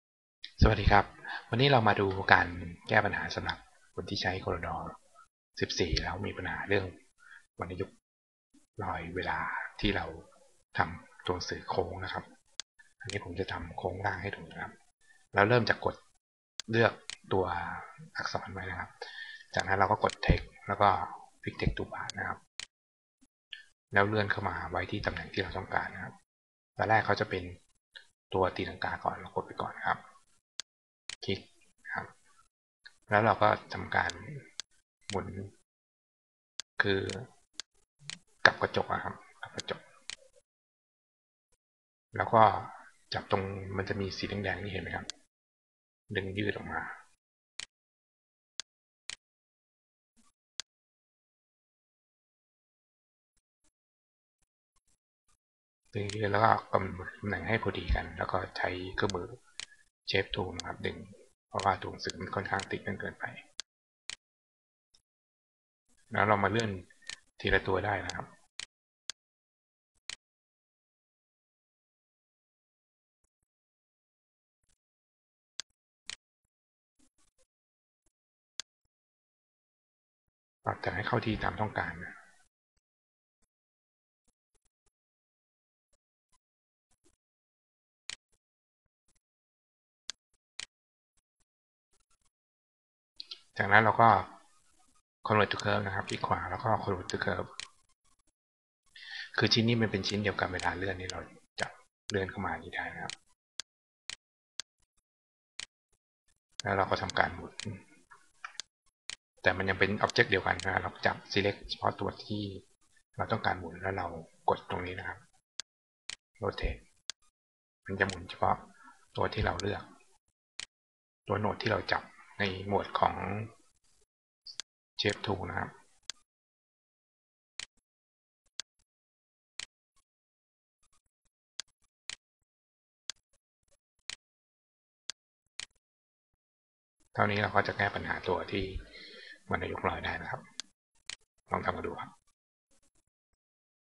457.วิธีแก้ สละลอย ตอนทำอักษรโค้ง ใน X4 ด้วยfit text to path
วิธีแก้ วรรณยุกติ์กระเด็น
เพื่อนสมาชิกหลายคนใช้วิธี พิมพ์แต่พยัญชนะก่อน แล้วทำโค้ง แล้วค่อยพิมพ์ วรรณยุกติ์เข้าไป แต่เราก็มีวิธี เลือนแล้วหมุนด้วย ฟังชั่น หมุนเฉพาะโหนดที่ select ใน shape tool  ทำให้การทำงานง่ายขึ้น ลดเวลาลง